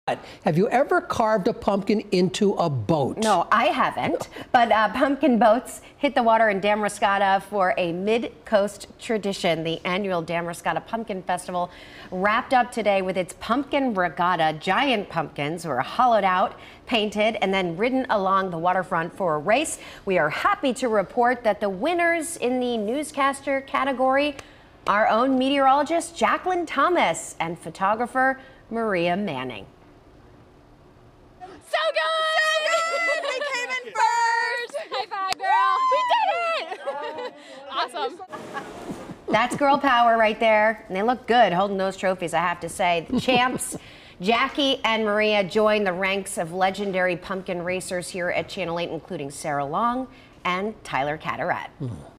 Maine pumpkinfest turns massive pumpkins into boats
The Damariscotta Pumpkinfest held its annual pumpkinboat regatta on Monday.